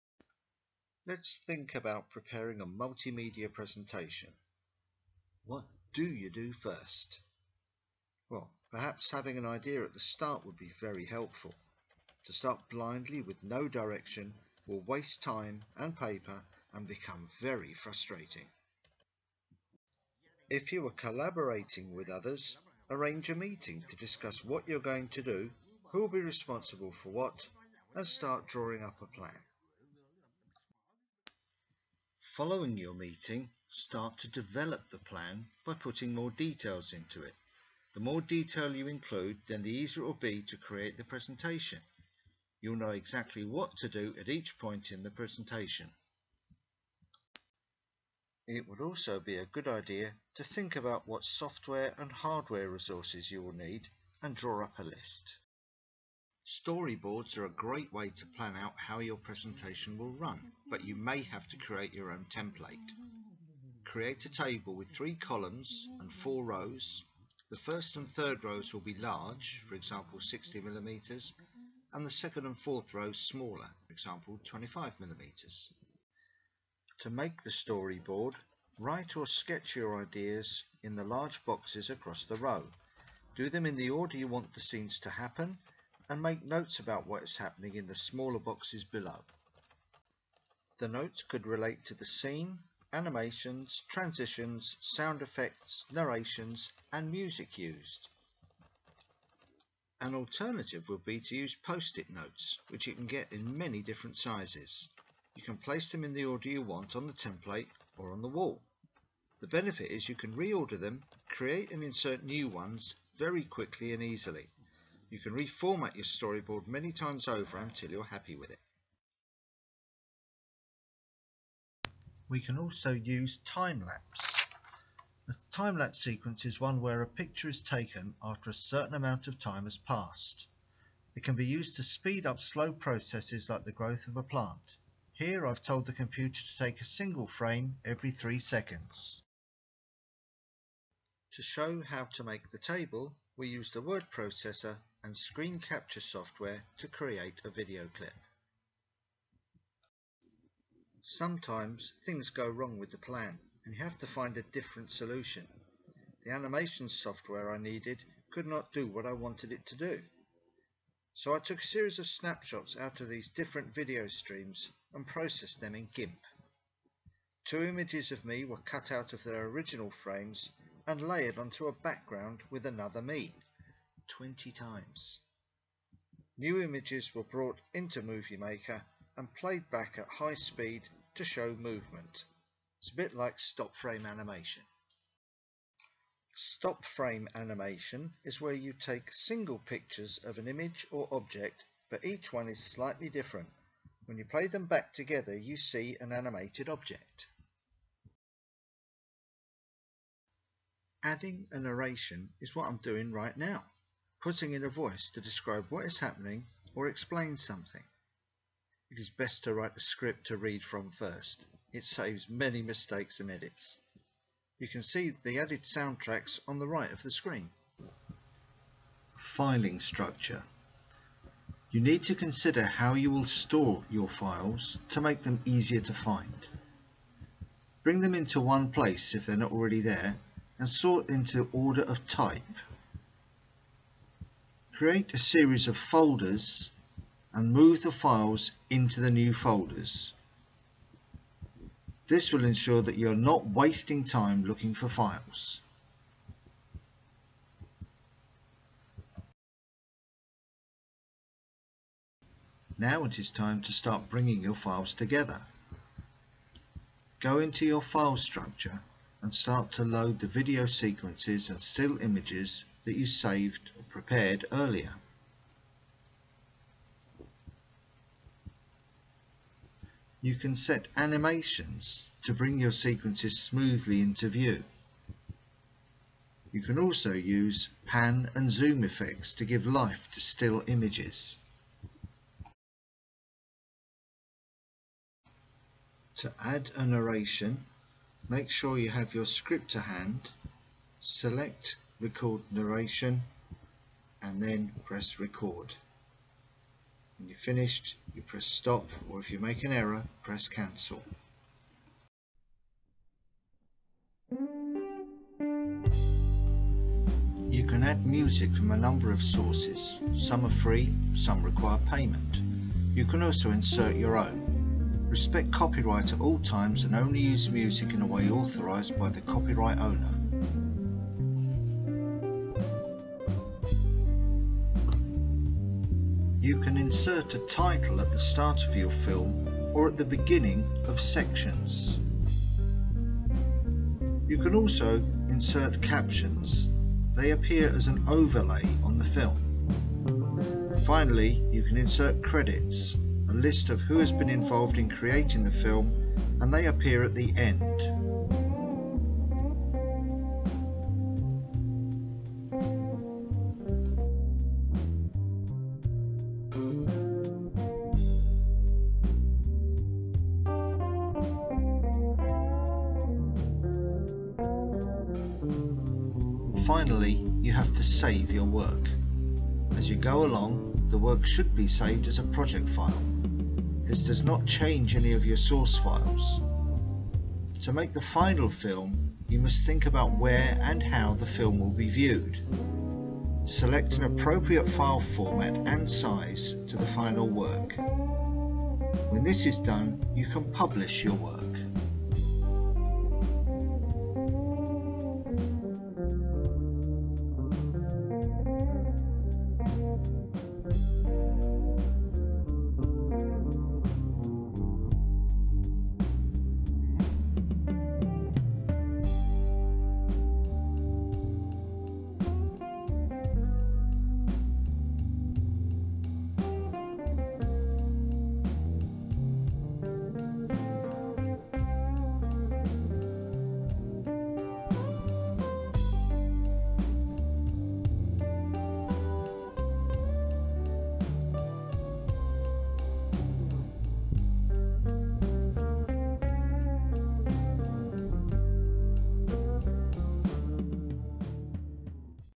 Martin's planning a film video
This was a film made to help students plan an animated film. It is a collection of my thoughts on what to do and reflects my experiments in animation using a variety of software tools. Might not be brilliant or indeed correct but I had enormous fun making it.